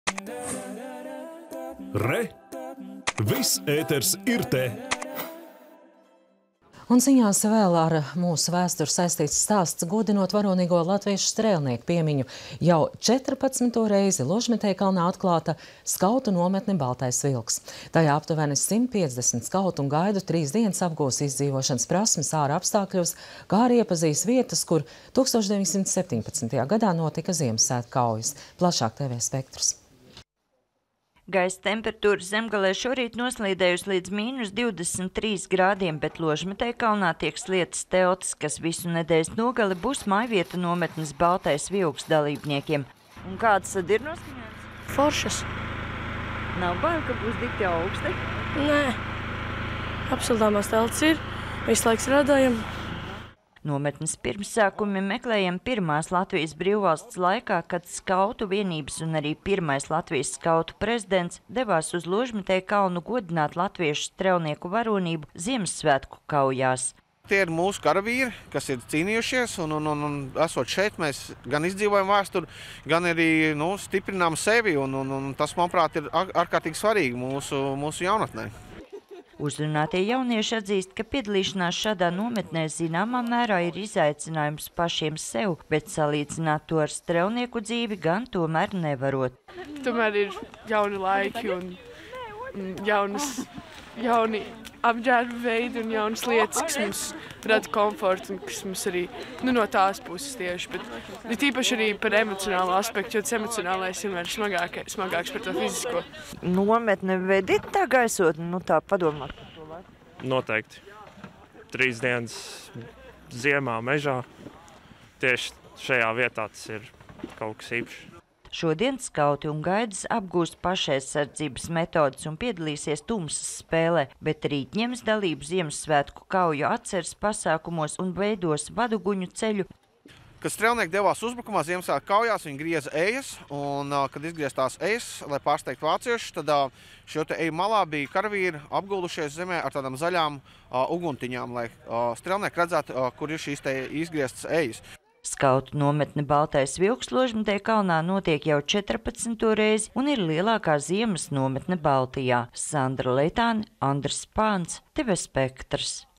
Skauti pulcējas ziemas nometnē “Baltais vilks”
Godinot varonīgo latviešu strēlnieku piemiņu, jau četrpadsmito reizi Ložmetējkalnā atklāta skautu nometne “Baltais vilks”. Tajā aptuveni 150 skautu un gaidu trīs dienas apgūs izdzīvošanas prasmes āra apstākļos, kā arī iepazīs vietas, kur 1917. gadā notika Ziemassvētku kaujas. Plašāk – TV Spektrs.